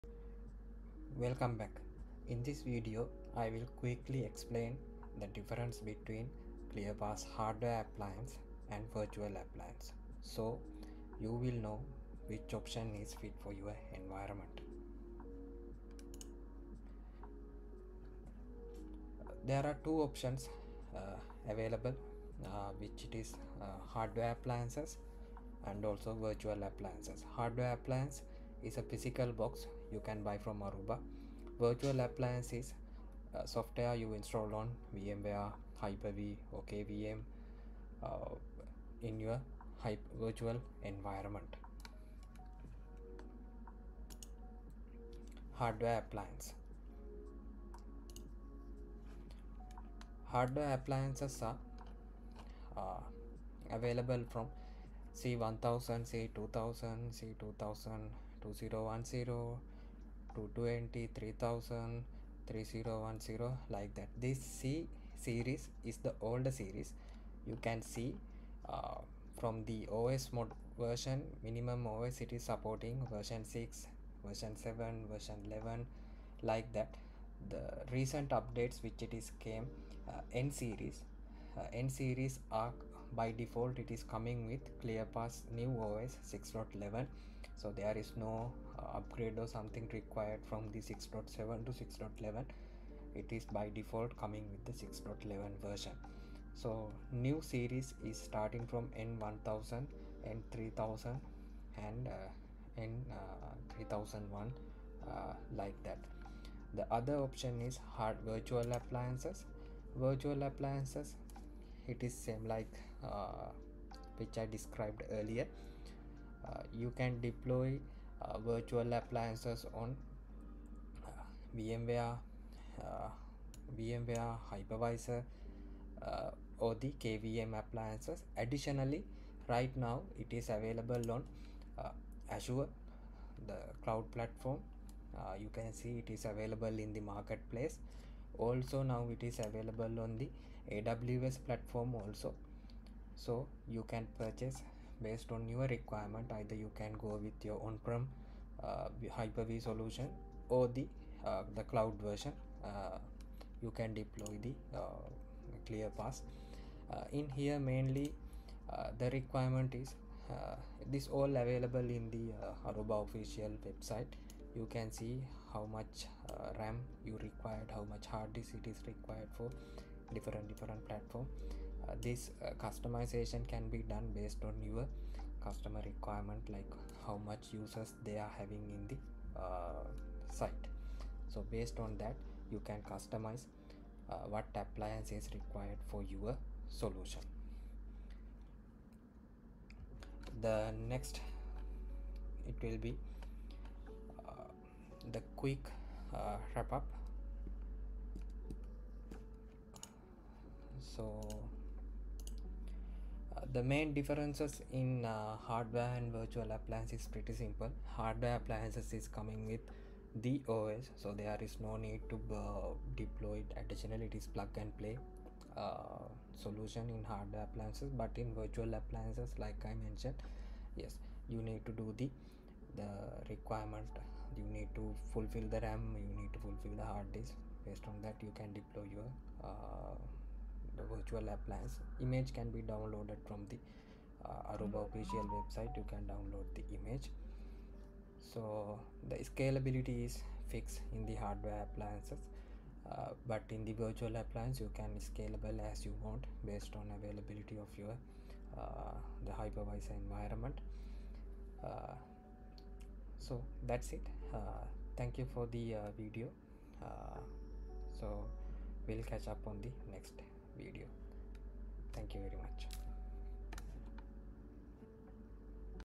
ClearPass Hardware vs Virtual Appliance | Aruba ClearPass Explained in 3 Minutes
Are you wondering whether to choose a ClearPass hardware appliance or a virtual appliance? 🤔
In this short video, I’ll break down the key differences, benefits, and use cases of each option — all in under 4 minutes.

✅ What you’ll learn:

What a ClearPass hardware appliance is

What a ClearPass virtual appliance is

Pros and cons of each deployment option

How to decide which one is right for your environment

Both hardware and virtual appliances deliver the same powerful Aruba ClearPass security features, but the choice depends on your IT strategy.